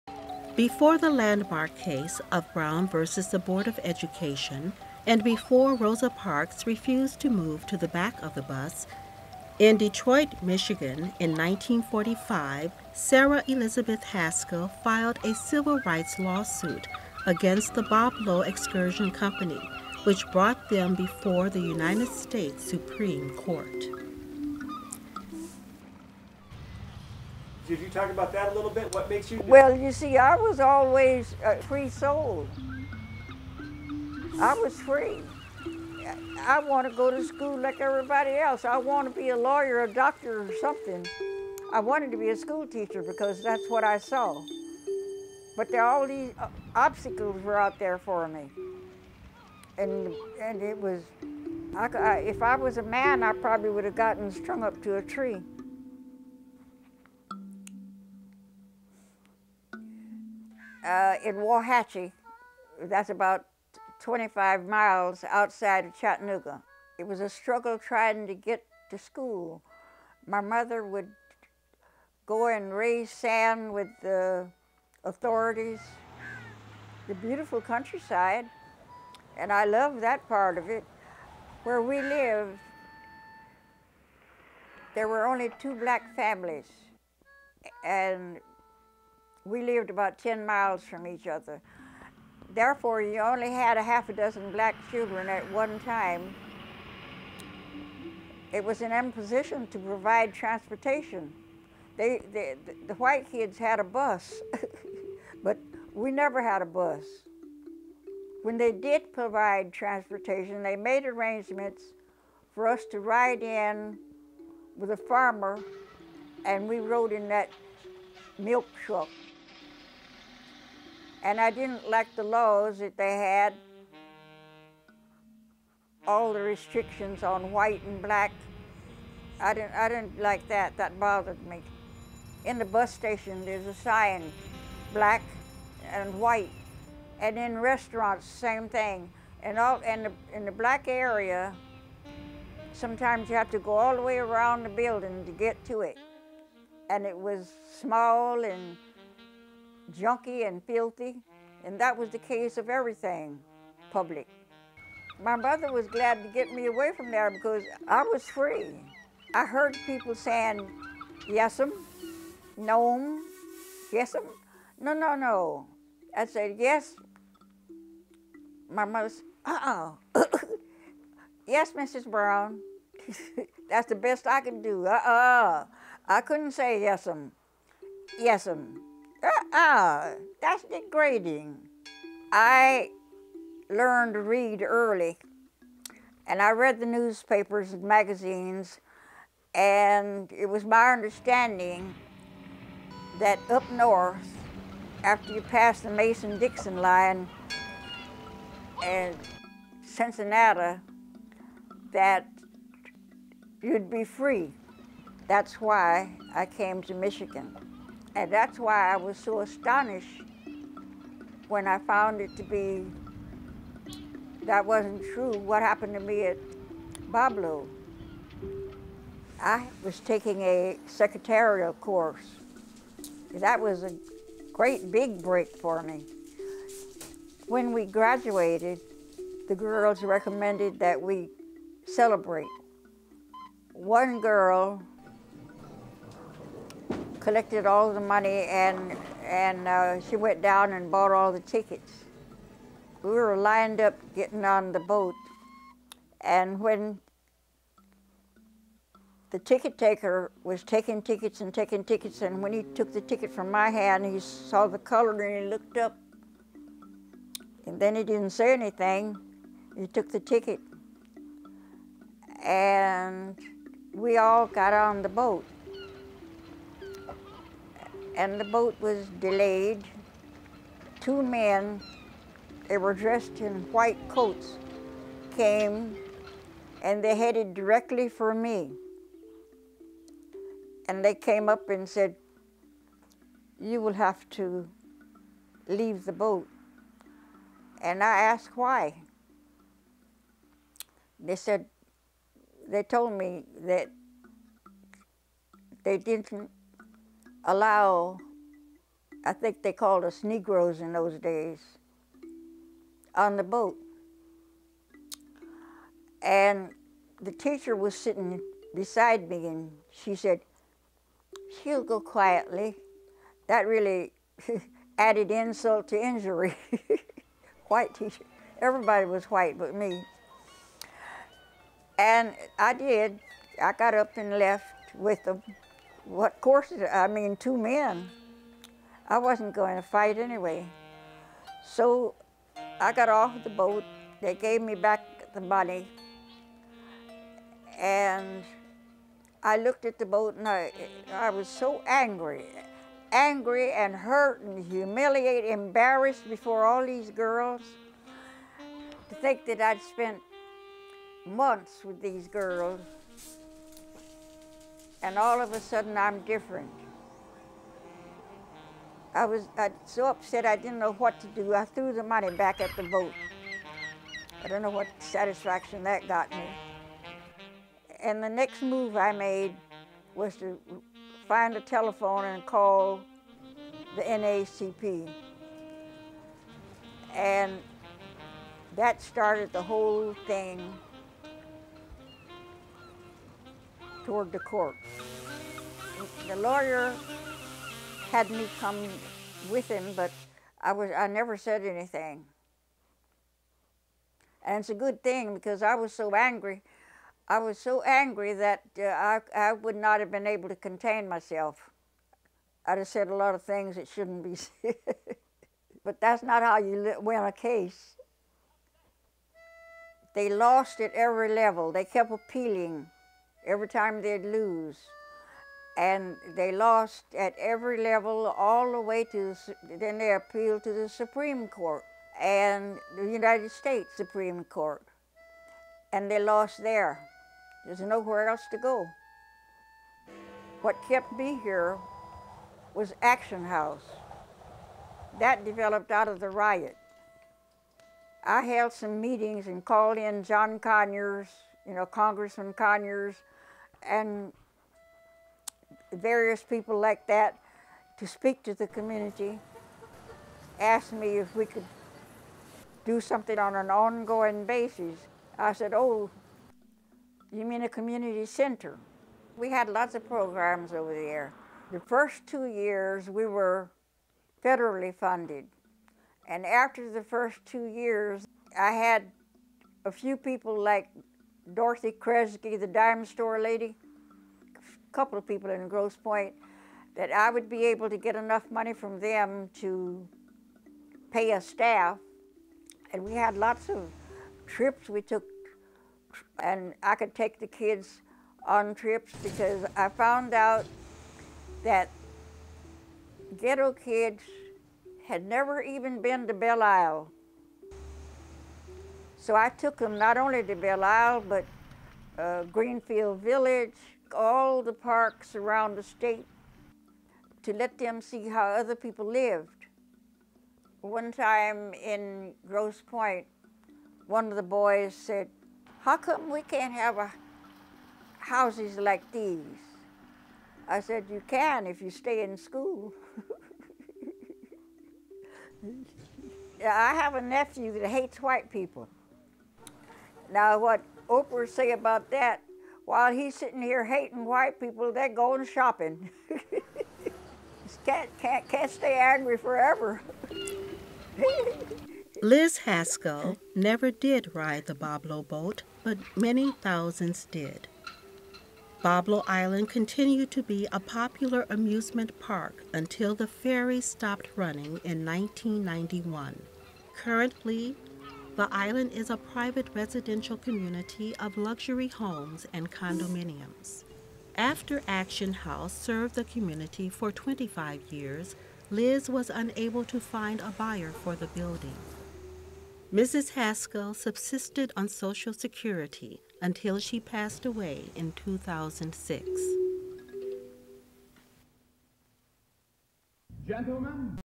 Rosa Parks of the Boblo Boat
Sara Elizabeth Haskell has been known as the "Rosa Parks of the Boblo Boat," but she actually led the way for Rosa Parks on the banks of the Detroit River in 1945.